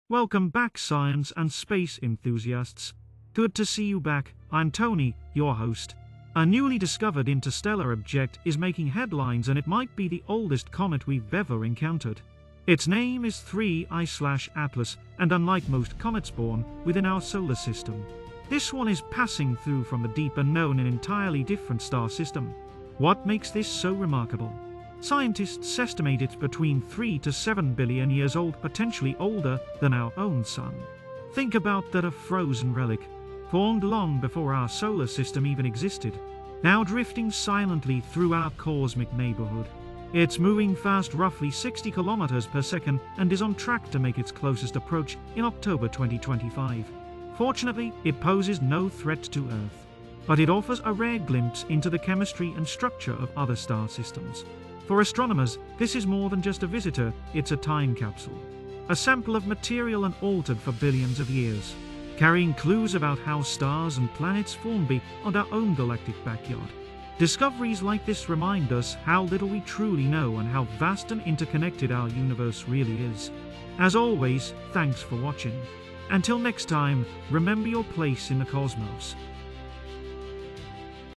This Ancient Comet Is Older Than the Sun!
🚨 BREAKING FROM DEEP SPACE: Scientists just found something older than the Sun flying through our solar system! 🌌

Say hello to 3I/ATLAS — a mysterious interstellar comet casually cruising past Earth like it owns the cosmos. 🛸 This isn’t sci-fi — it’s a real space rock that could be over 7 billion years old. Yeah… billion with a “B.” 🤯

In today’s episode, I’ll show you what makes this icy traveler so mind-blowingly rare, how fast it’s speeding through the stars, and why scientists are absolutely LOSING it over this discovery. 🚀

If you're into space mysteries, ancient cosmic relics, or just wanna impress your friends with brainy facts, this one’s for you. 👽✨

👇 Drop a comment:
What do YOU think 3I/ATLAS is hiding from us?
Alien surveillance drone? Space fossil? Just passing through?